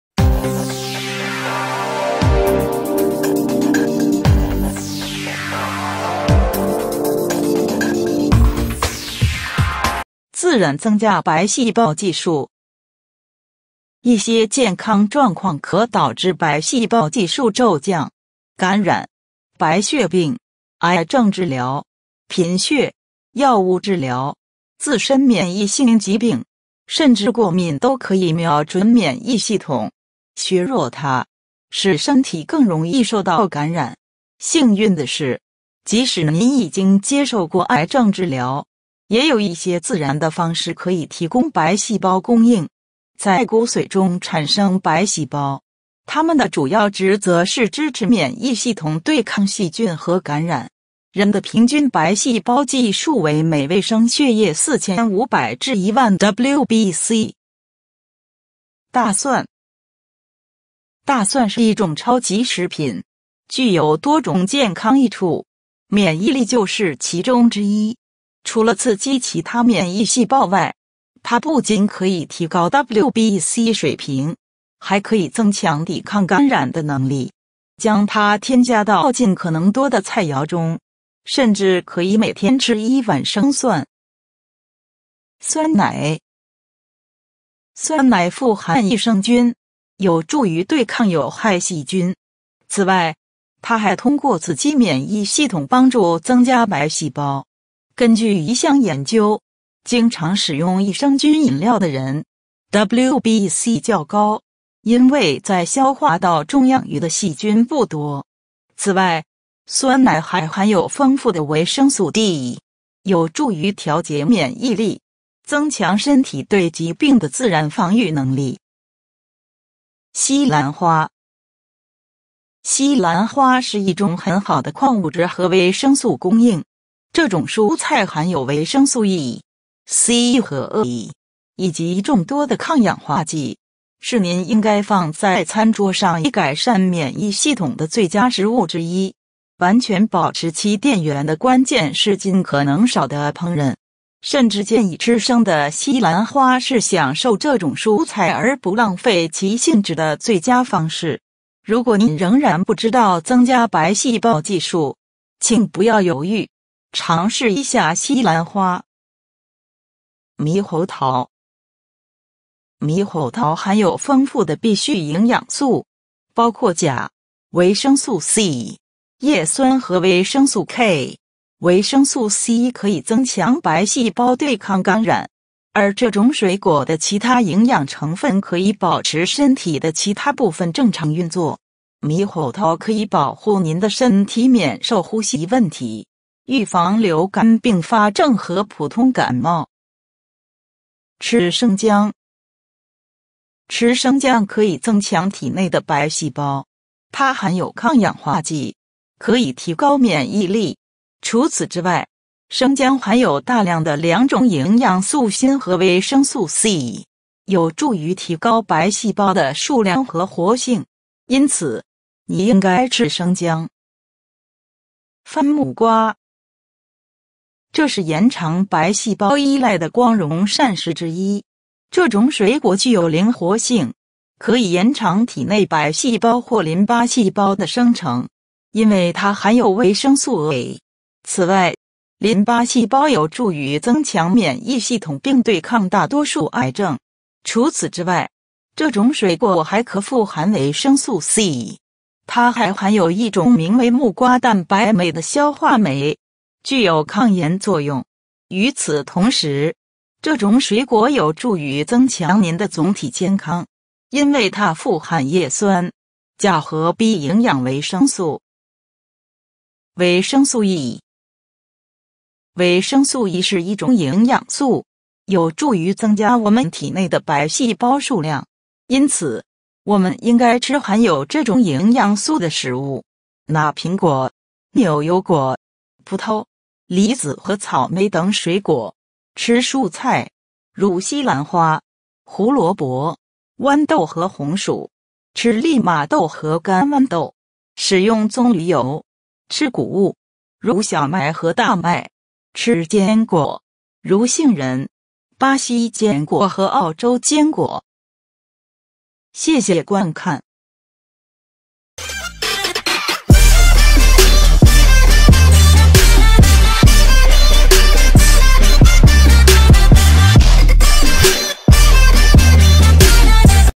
自然增加白细胞计数
一些健康状况可导致白细胞计数骤降。感染，白血病，癌症治疗，贫血，药物治疗，自身免疫性疾病，甚至过敏都可以瞄准免疫系统，削弱它，使身体更容易受到感染。幸运的是，即使您已经接受过癌症治疗，也有一些自然的方式可以提供白细胞供应。在骨髓中产生白细胞。他们的主要职责是支持免疫系统对抗细菌和感染。人的平均白细胞计数为每微升血液4,500至10,000 WBC。